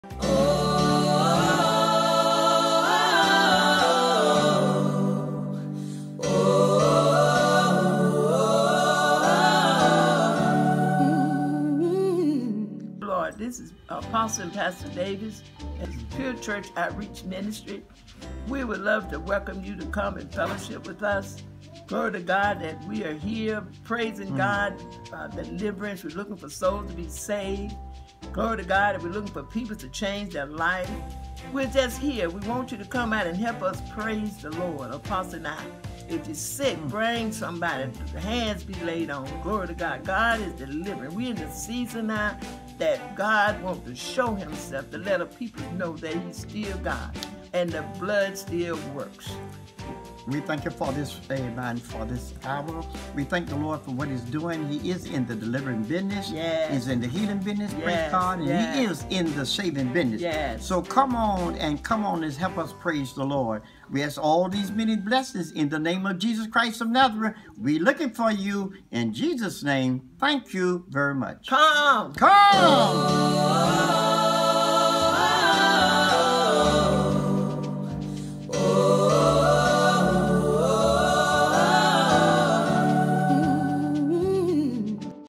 Pure Church Invitation
Pastor & Apostle Davis invites you to join them at Pure Church Outreach Ministry.